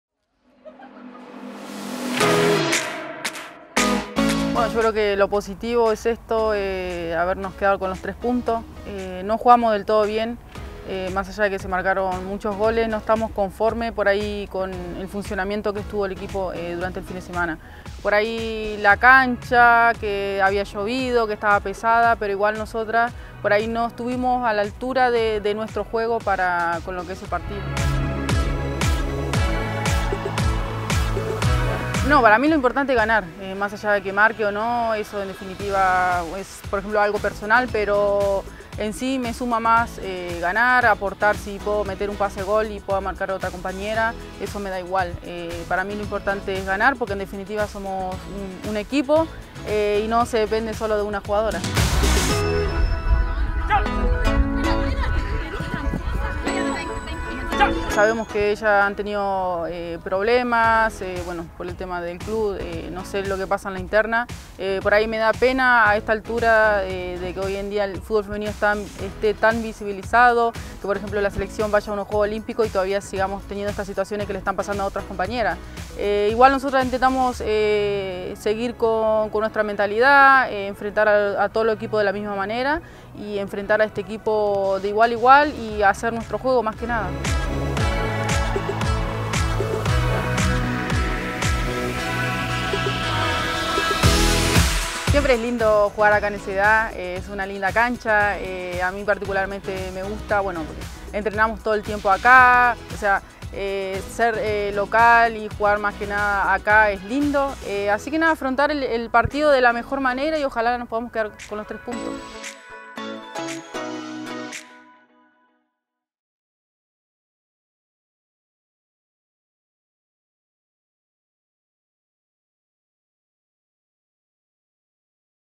Yael Oviedo y el duelo ante Antofagasta: "Vamos a enfrentarlo de la mejor manera"
Este domingo, el Primer Equipo femenino azul disputará la segunda fecha del Campeonato Femenino Caja Los Andes 2021, donde recibirá a Deportes Antofagasta en el Centro Deportivo Azul. Previo al encuentro ante las "Pumas", la delantera argentina Yael Oviedo, comentó sus sensaciones tras el debut y señaló cuáles serán las armas del equipo en este torneo. "Nosotras intentamos seguir con nuestra mentalidad, enfrentar a los equipos de la mejor manera, de igual a igual y hacer nuestro juego". Revisa la entrevista a continuación.